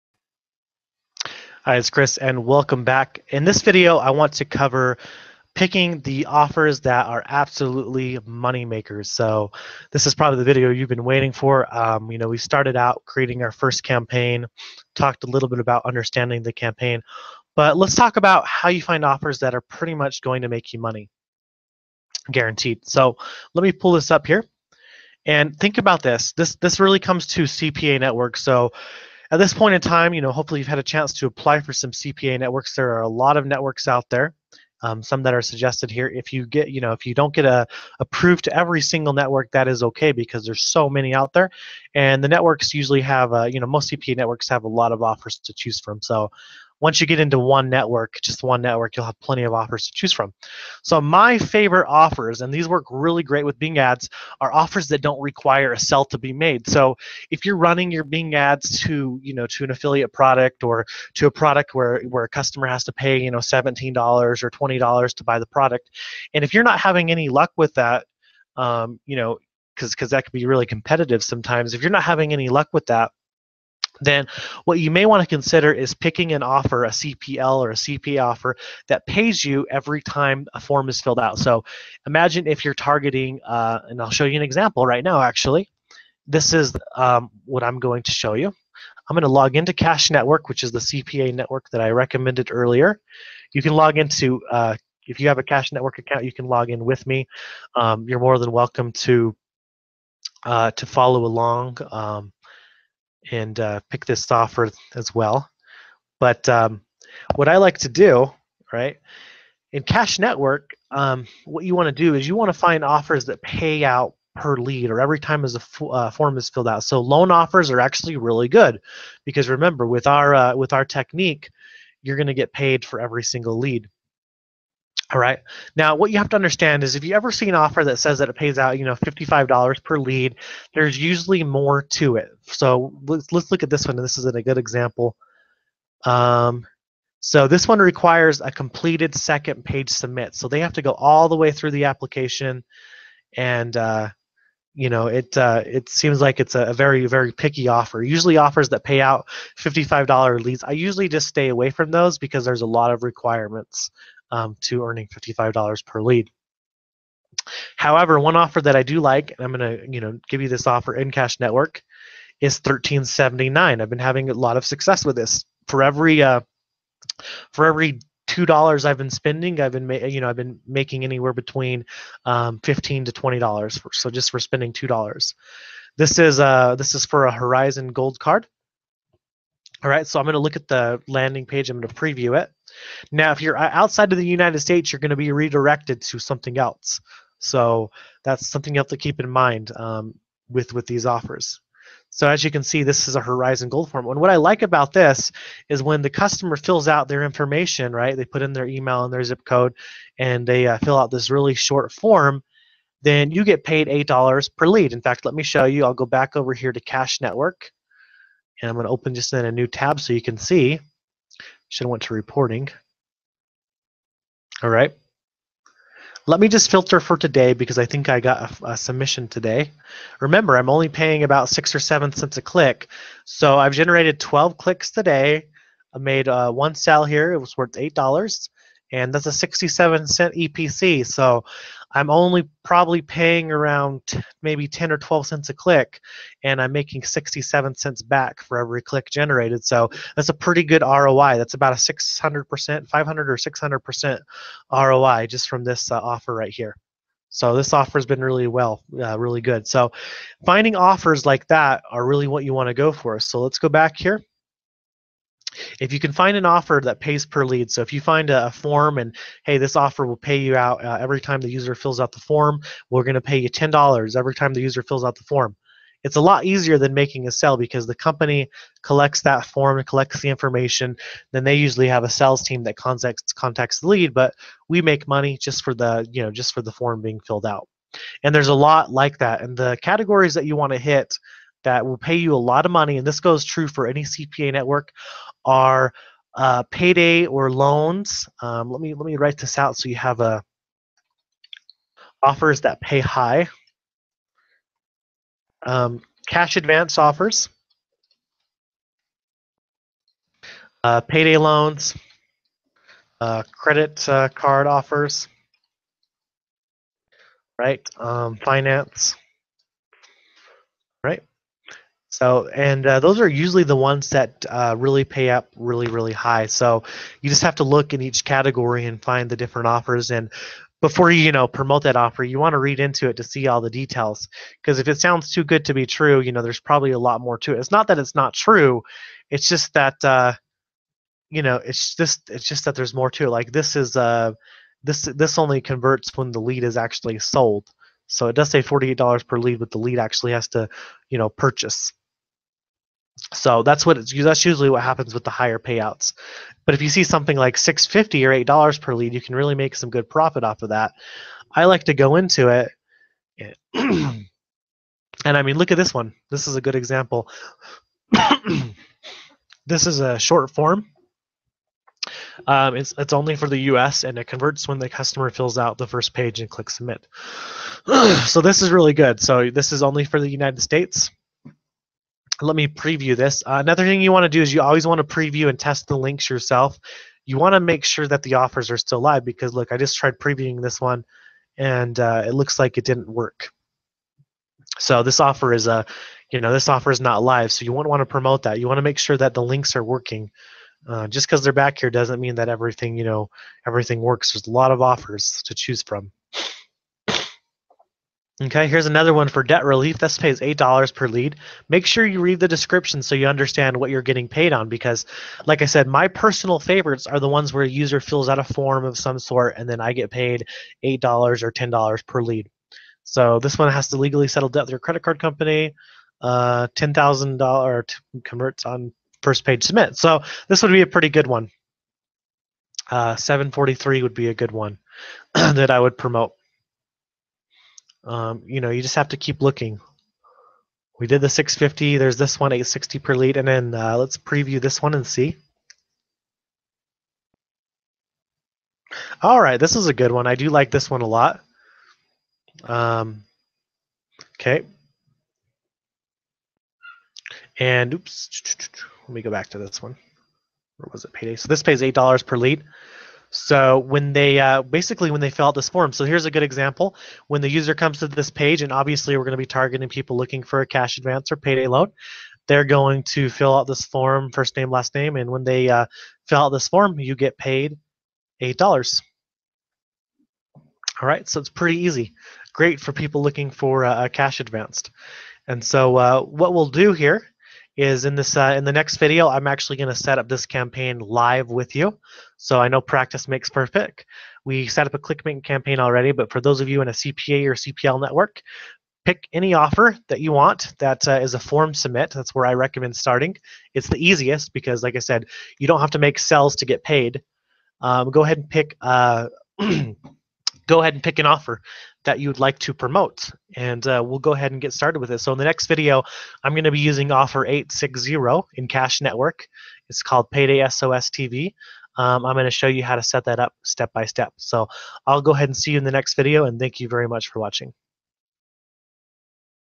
Pay Per Lead Offers | Bing CPA Training - Step 7
The Keywords opportunities menu under Tools provides the following three areas of functionality:

Keyword suggestions to help expand keyword or negative keyword lists
Traffic history and demographic data to help target the most relevant audience
Bid suggestions, for improved ad impressions and click-through rates
Let’s take a closer look at how these feature groups work.

Keyword research

With Bing Ads Editor, you can discover new keywords and bids using the keyword research tools, then easily add them to your campaigns.

From the account, campaign or ad-group level, select a keyword and choose Find related keywords. A list of similar keywords appears with additional performance data such as impressions, CTR and CPC. If you want keywords that match the language on a particular website, click Get keywords from a website and enter a URL.

Either way, make your selections by clicking the check boxes next to the terms. In the Add selected keywords drop-down menu, choose whether to add them as ad group keywords, a campaign or ad group negative keywords. As always when using Bing Ads Editor, remember to sync your changes to make them active.

Traffic and demographic research

When you select a keyword and choose Show traffic history, you are given that keyword’s search data from the previous three months, and an estimate of its usage for the current month. This information can help you improve your bidding strategy. For example, you might want to increase your bid during the months a keyword performs better.

Click Age demographic to display the percentage of keyword searches across six age ranges. Clicking Gender demographic displays the percentage of keyword searches made by males, females and unspecified gender. You can use age and gender data to increase ad delivery to your target audience. For example, you might use incremental bids to target a gender that searches more often for the keywords in an ad group